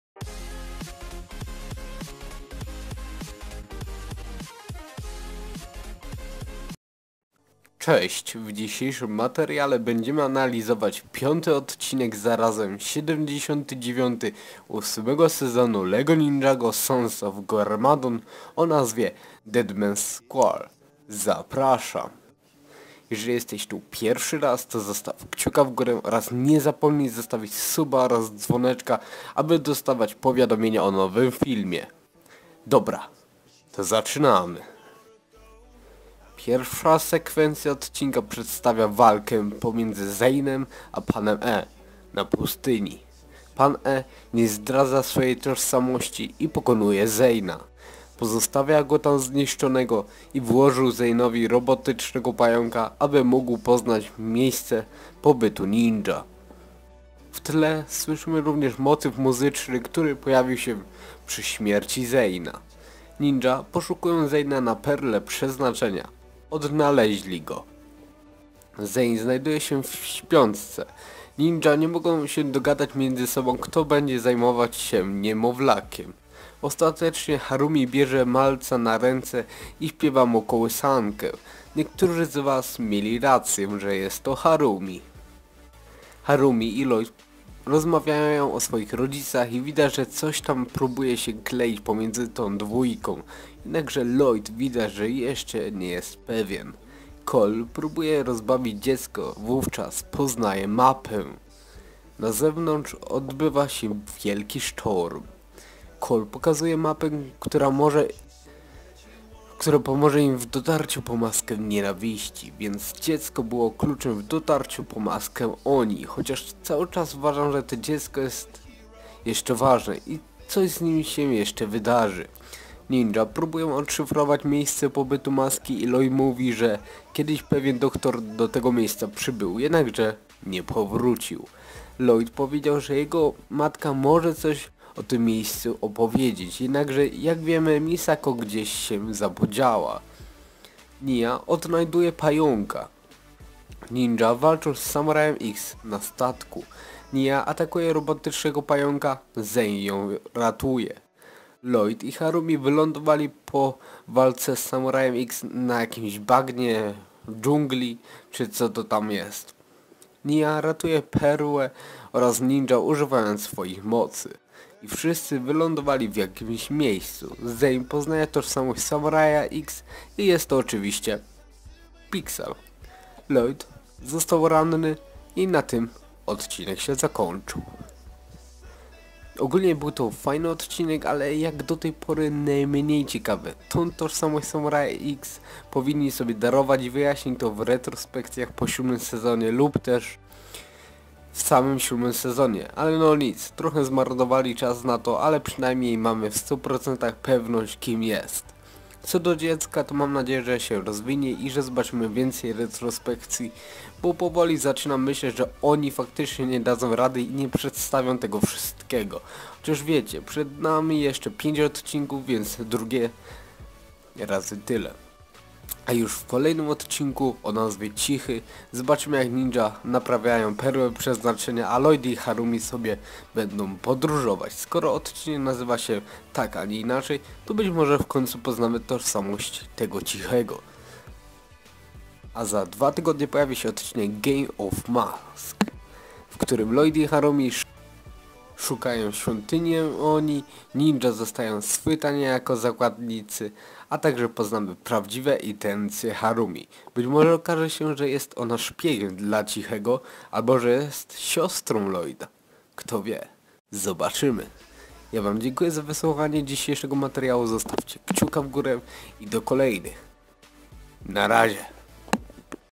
ANALIZA 79 ODCINKA 8 SEZONU LEGO NINJAGO SONS OF GARMADON!
Cześć! W dzisiejszym odcinku analizujemy sobie 5 odcinek o nazwie ""Dead Man's Squall" z 8 sezonu LEGO Ninjago Sons of Garmadon! Zapraszam! :)